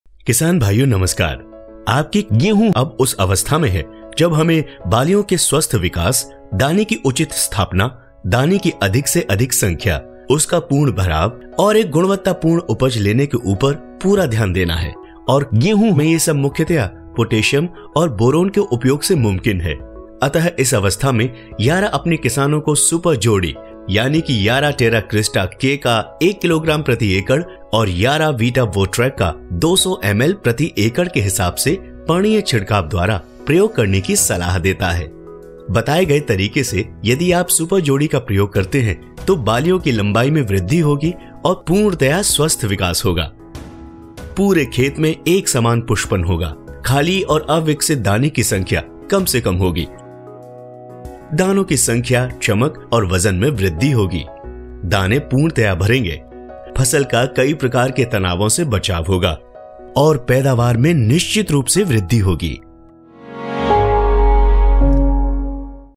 पाए गेहूं की स्वस्थ बालियॉं Yara की सुपर जोड़ी के साथ
Yara की सुपर जोड़ी का प्रयोग करने से गेहूं में बालीयों की लम्बाई में वृद्धि होगी और पूर्णतया स्वस्थ विकास होगा।